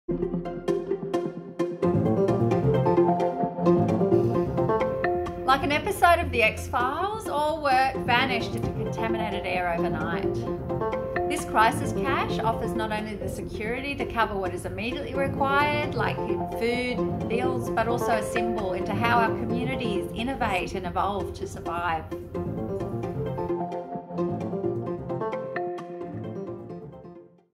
1000 x 1000: Crisis Cash Recipient Glitta Supernova
1000 x 1000: Crisis Cash Recipient Glitta Supernova shares the impact of receiving emergency funding. 1000 x 1000: Crisis Cash is a one-off emergency payment to support INDEPENDENT ARTISTS IN THE PERFORMING ARTS who have been significantly affected by COVID-19 in Australia and who do not have access to a financial safety net. We seek to ensure artists are able to continue their practice while dealing with the day to day impacts of COVID-19 on their lives.